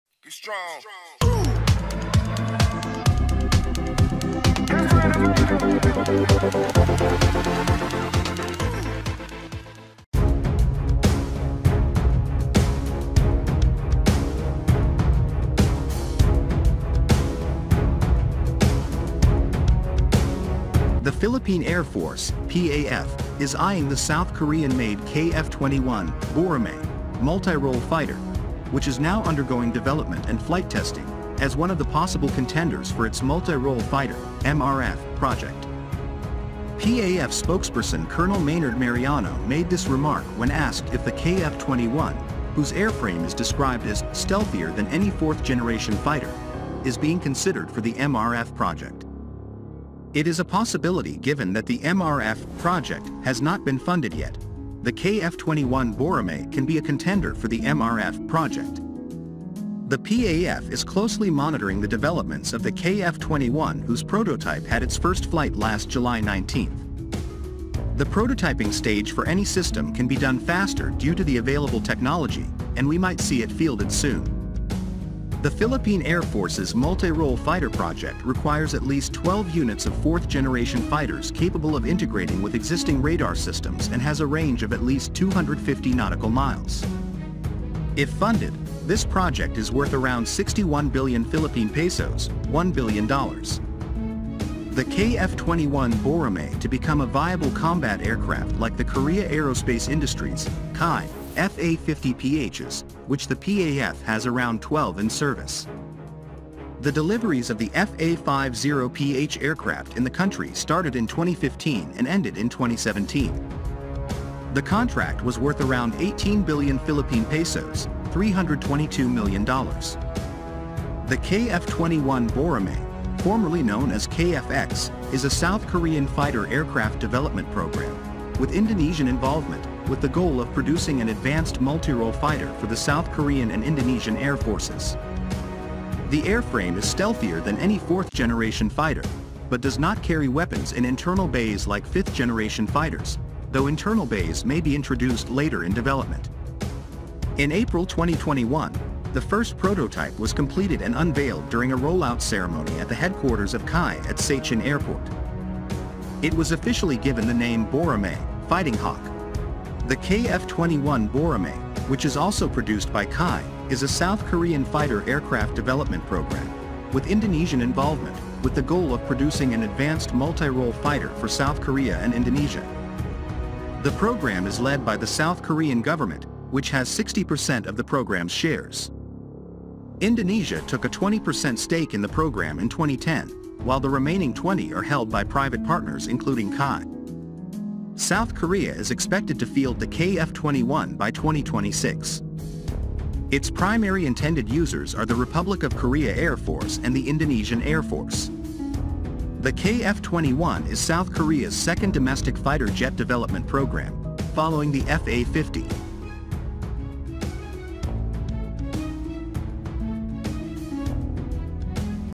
The Philippine to Purchase South Korean-made KF-21 "Boramae", which is more advanced than the Gripen
The Philippine Air Force (PAF) is eyeing the South Korean-made KF-21 “Boramae” multi-role fighter, which is now undergoing development and flight testing, as one of the possible contenders for its multi-role fighter (MRF) project. PAF spokesperson Col.

Maynard Mariano made this remark when asked if the KF-21, whose airframe is described as “stealthier than any fourth-generation fighter” is being considered for the MRF project. It is a possibility given that the MRF (project) has not been funded yet, the KF-21 Boramae can be a contender for the MRF (project). The PAF is closely monitoring the developments of the KF-21 whose prototype had its first flight last July 19.